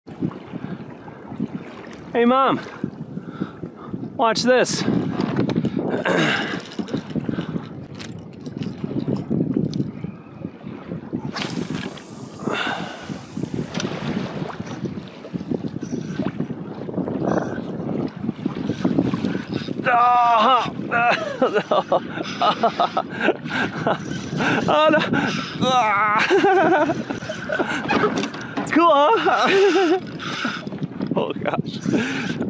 Called My Shot! Kayak Fishing for Topwater TUNA!
Angler accidentally catches the biggest grouper species on Earth!